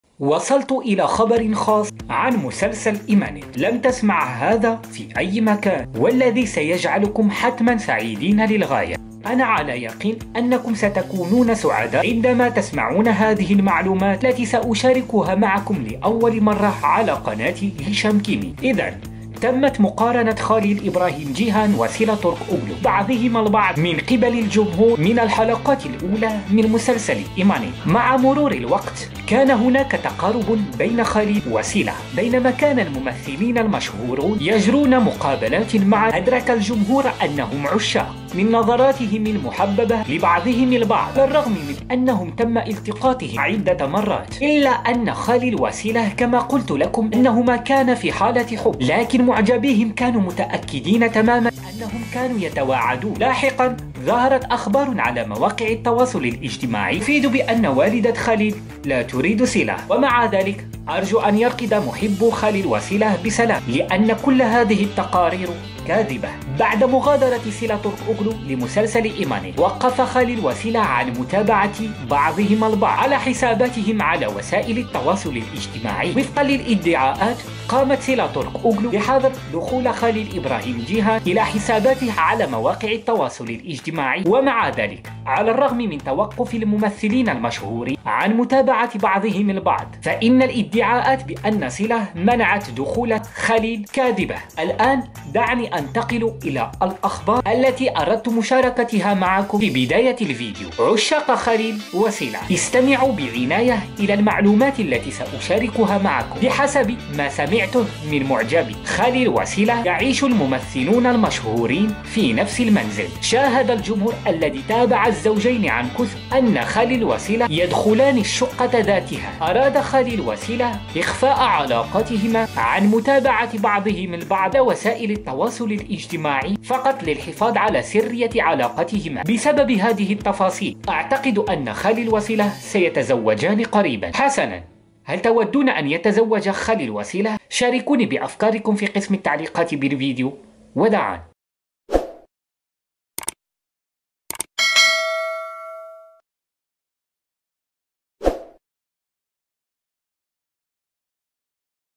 وآخيرا هذه هي حقيقةً 😱 إلغاء متابعة خليل و سيلا تورك لبعضهما البعض 😞 " فعلوها لإخفاء سرية حبهما "
تعرف على قصة الممثلة الجميلةً اوزجي ياغيز بطلة مسلسل " أبي " 🇹🇷  كيف تعيش حياتها الواقعية ؟